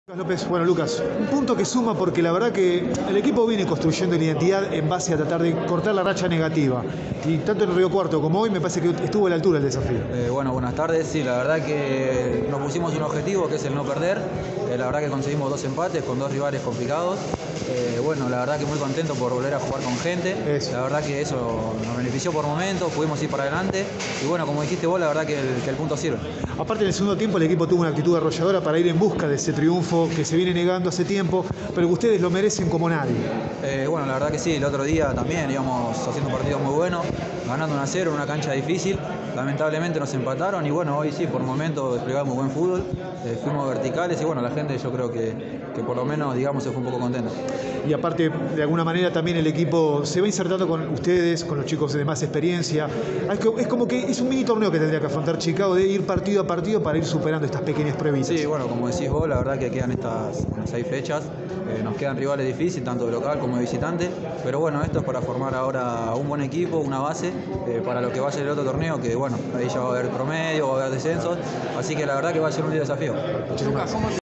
Lucas López (Nueva Chicago 0 - Temperley 0)
La palabra de la figura del volante de Chicago luego del partido